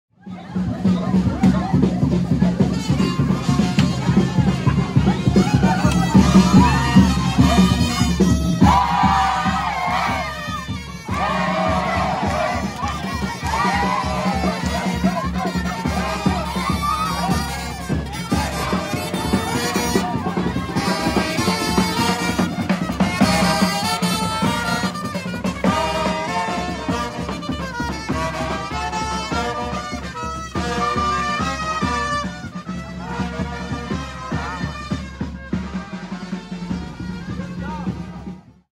Celebrating Eden Leadership
Eden Projects employees from the village of Bemangoraka parading through our community to say thank you to National Director, Josy.